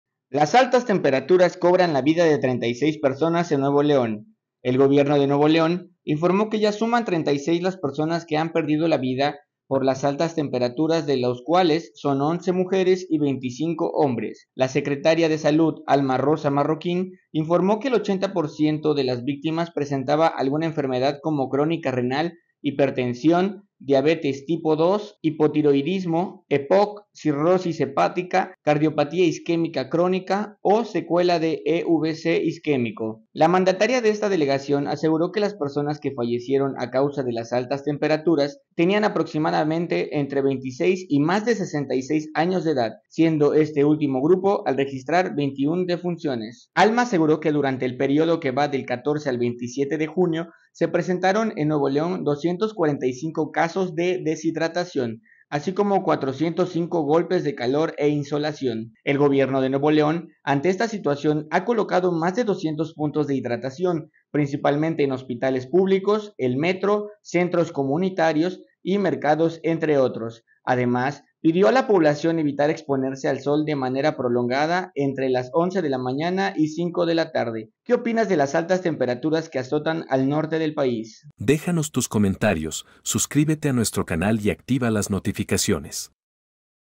Altas temperaturas provocan decesos en todo México
El gobierno de Nuevo León reporta 36 pérdidas de vida por las altas temperaturas que se presentan en el norte del país, sin embargo para contrarrestar esta situación a colocado más de 200 puntos de hidratación.